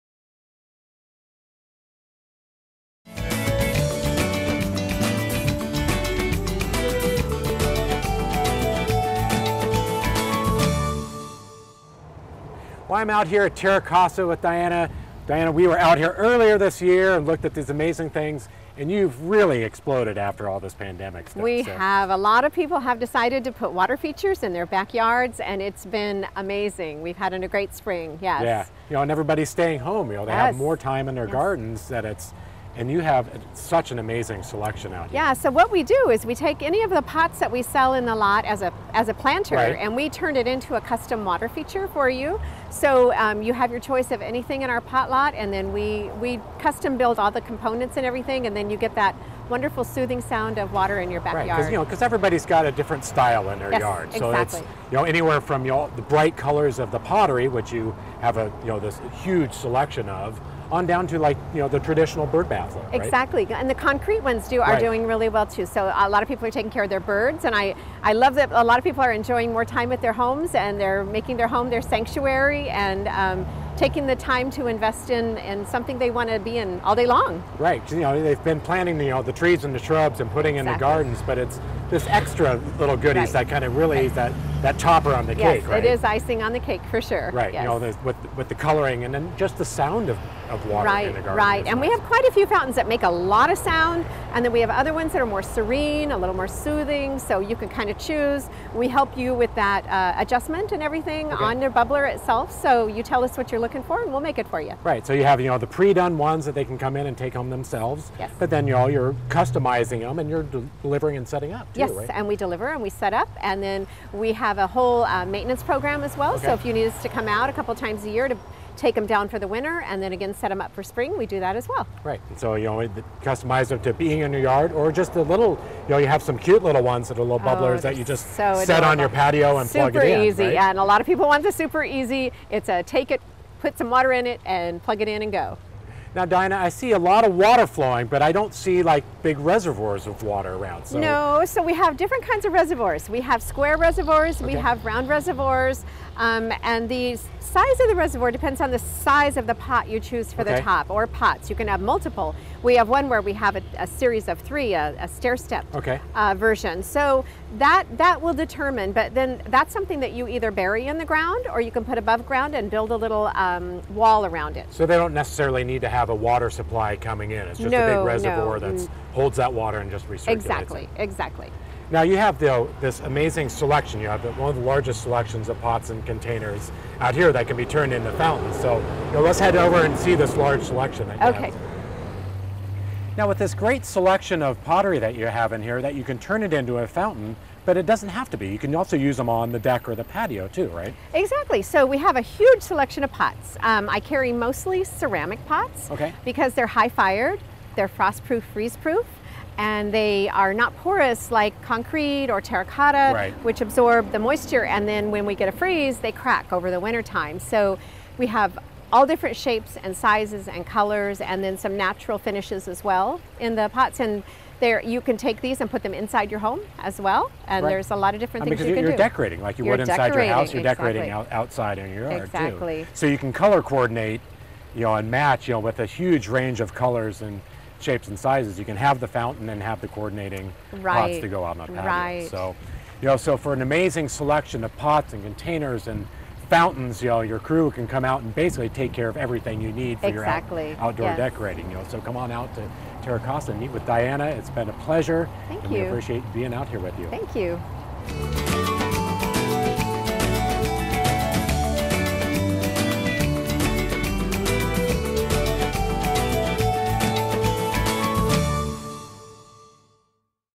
Terra Casa Summer Fountains
With a lot of people staying home this summer, people are looking to build relaxing spaces in their garden.  Fountains are a great way of doing that.  We check out some options.